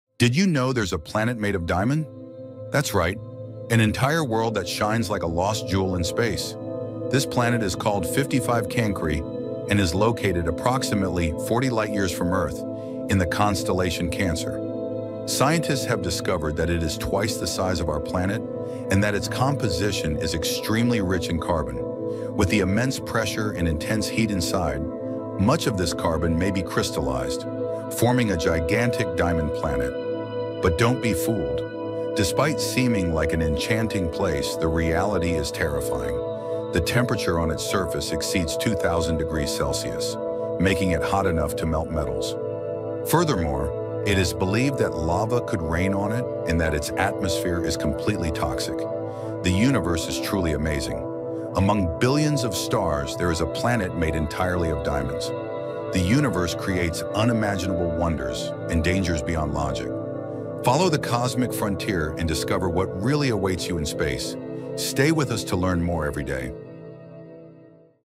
A Planet Made of Diamond — But You’d Never Survive It
Did you know there's a planet made of diamond? It's called 55 Cancri e, located about 40 light-years from Earth, and it’s one of the most extreme worlds ever discovered. Despite shining like a cosmic jewel, its surface reaches over 2000°C, hot enough to melt metal, with a toxic atmosphere and possible lava rain. The universe is breathtaking—and terrifying.
Keep exploring the wildest mysteries of the cosmos with The Cosmic Frontier.